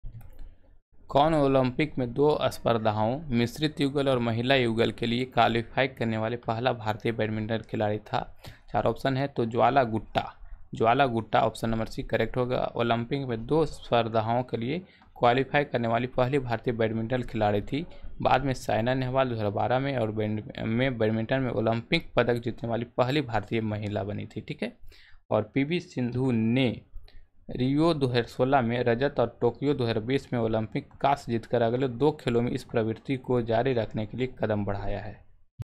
कौन ओलंपिक में मिश्रित युगल और महिला युगल के लिए क्वालीफाई करने वाला पहला भारतीय बैडमिंटन खिलाड़ी था
kaun olampik mein mishrit yugal aur mahila yugal ke lie kvaaleephaee karane vaala pahala bhaarateey baidamintan khilaadee tha
Who was the first Indian badminton player to qualify for mixed doubles and women's doubles at the Olympics?

writeralok